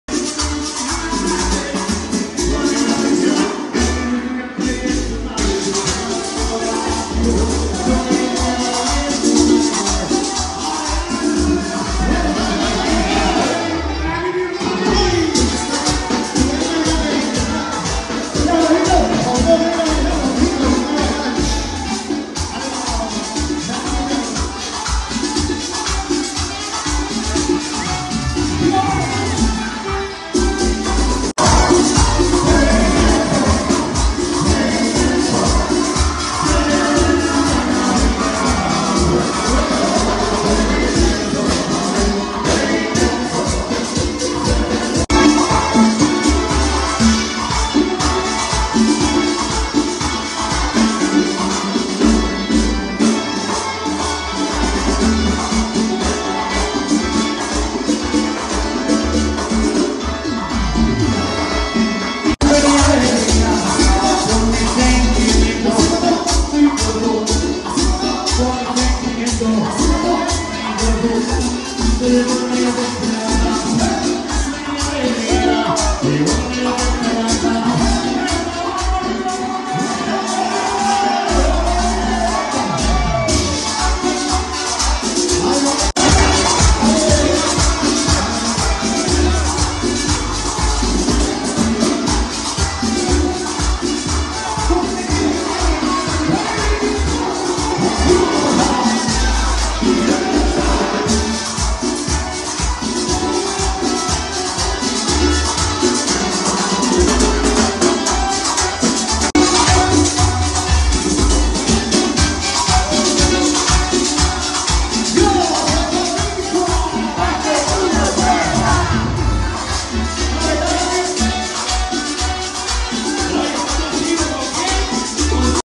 Cumple De Valerie Raquiti
📍Parte De Lo Que Sucedió Ayer Domingo En El Cumple De Valerie raquiti El Cual Se Lo Disfrutó De Principio A Fin, Todos Los Presentes Gozaron En Cantidad.